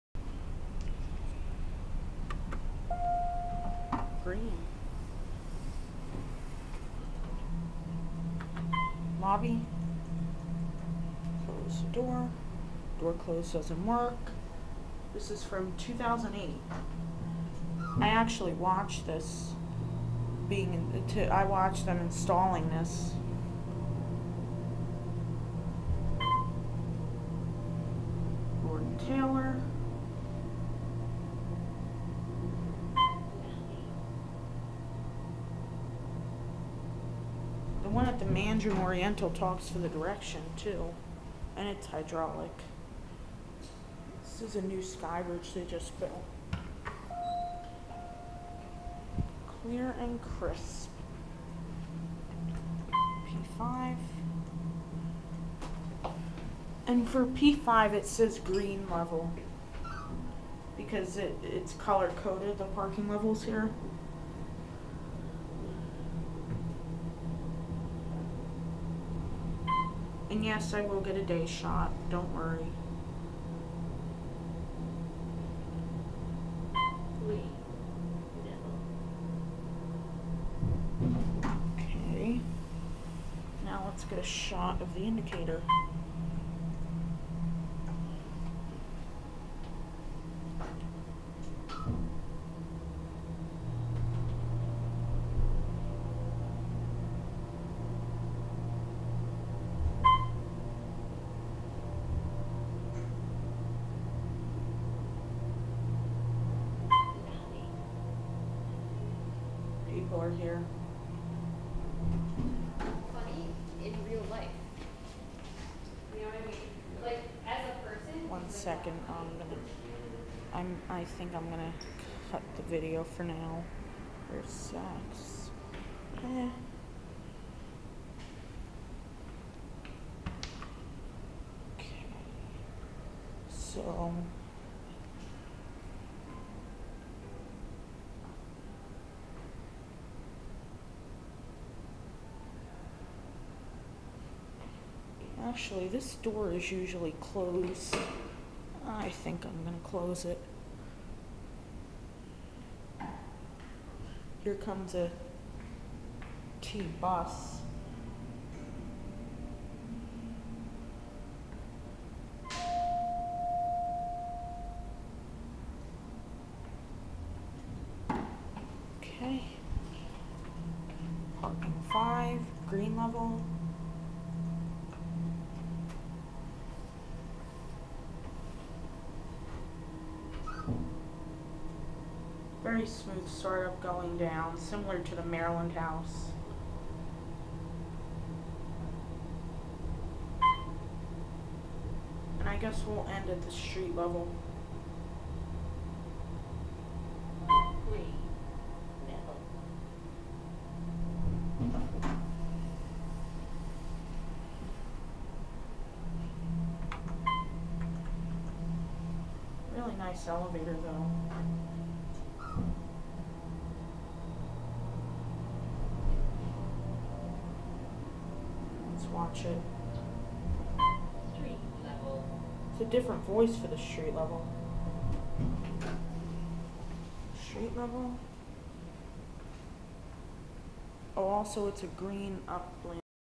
Otis 2000H Hydraulic Elevator in Newbury Arcade - The Shops at Prudential Center
Installed in 2008, Inground Hydraulic, ~150fpm, I will get a day shot in the next 8 months